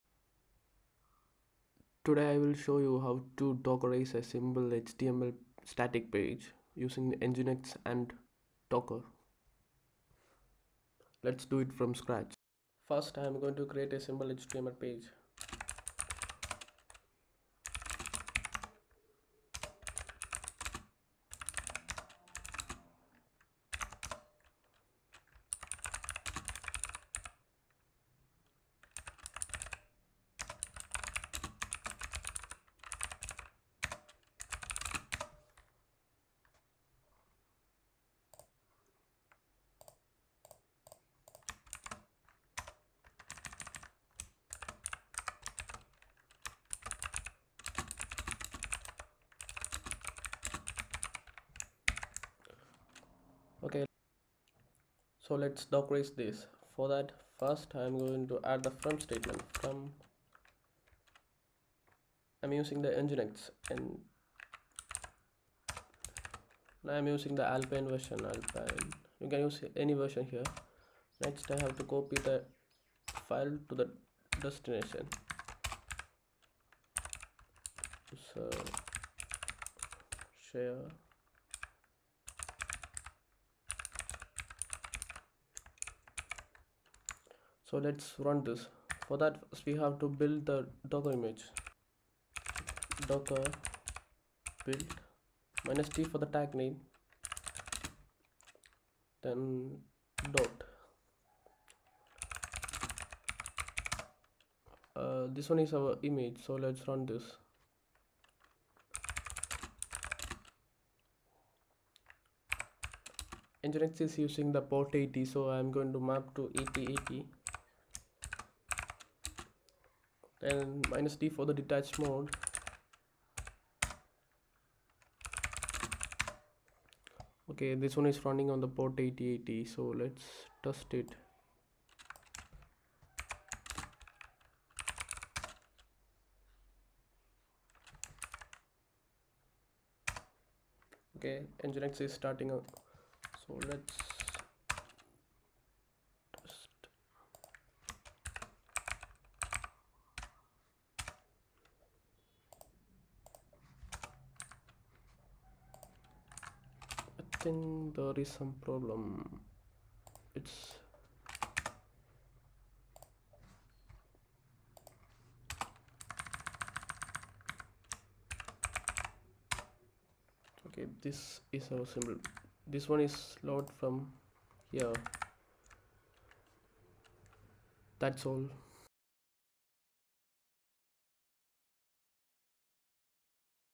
Containerizing a static HTML page using nginx and docker | DevOps
To containerize a simple HTML page.

This is the basic for hosting front-end technologies like react.js,next.js etc.Making the build and host the HTML output through docker.